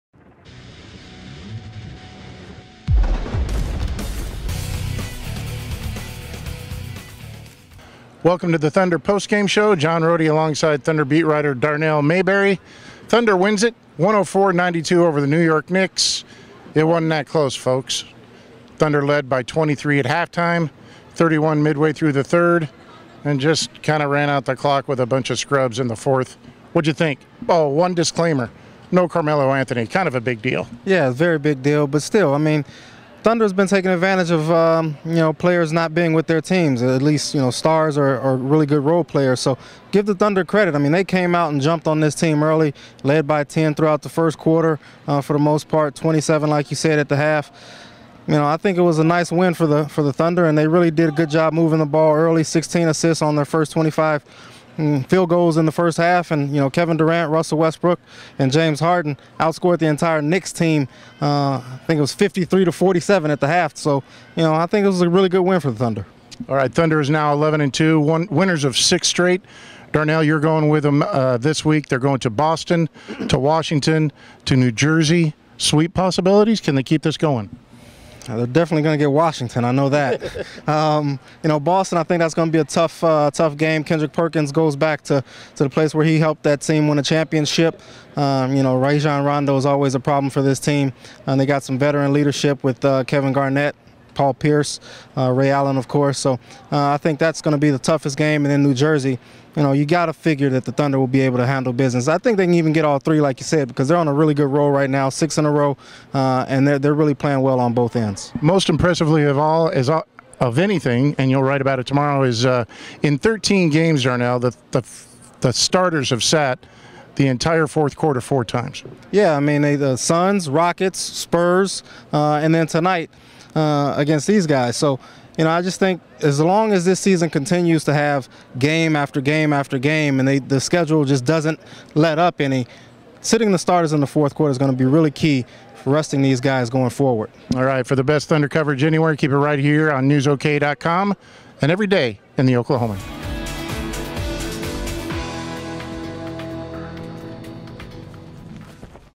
Analysis: Thunder-Knicks (2012-01-15)
Darnell Mayberry and John Rohde examine OKC's 104-92 dismantling of the New York Knicks on Saturday night at Chesapeake Energy Arena.